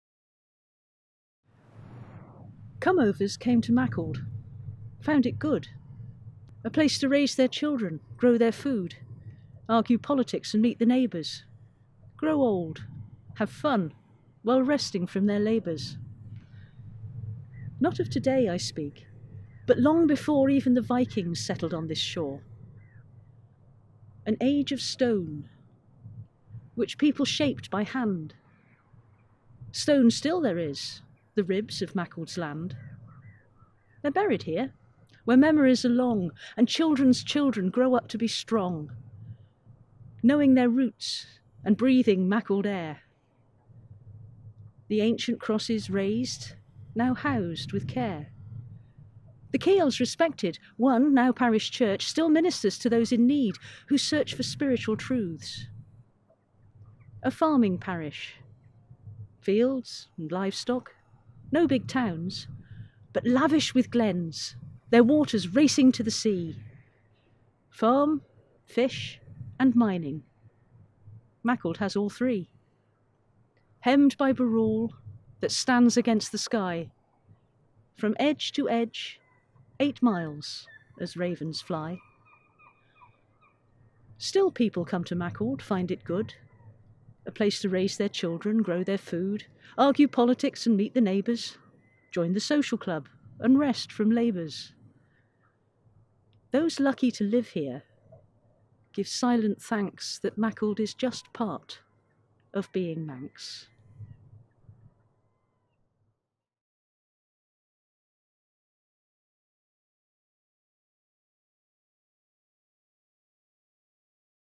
Top right-hand corner: A poem by Sara Goodwins
A poem by Sara Goodwins about the parish of the Isle of Man in which she lives, Maughold.

This film was created as a part of the Poetry Map of the Isle of Man project celebrating the Isle of Man in poetry.
This project is organised by Biosphere Isle of Man with support from Culture Vannin.